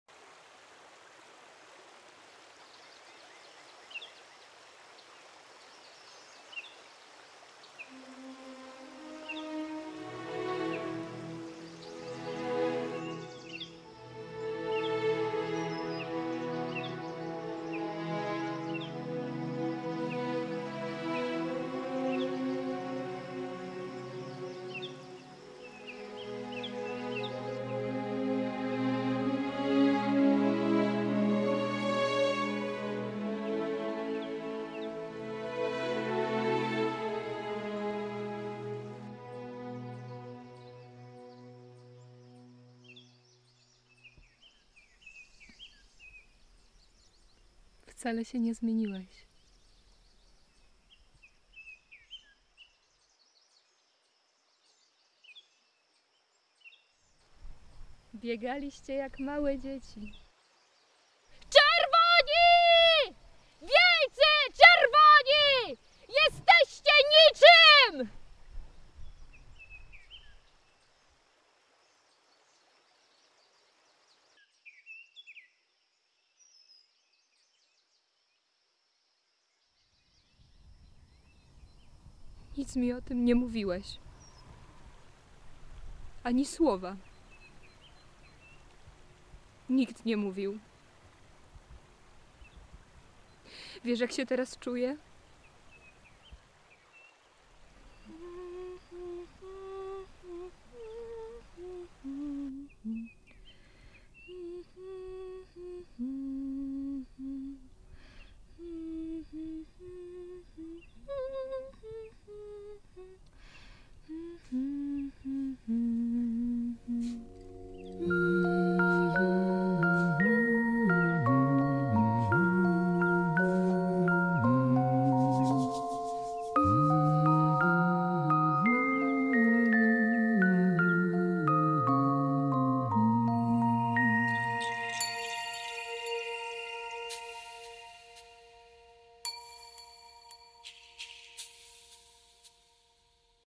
Über alle Ufer - Short Film Exercise
Warsaw Film School Erotica Exercise - Cinematography
Shot On Atlas Orion Anamorphics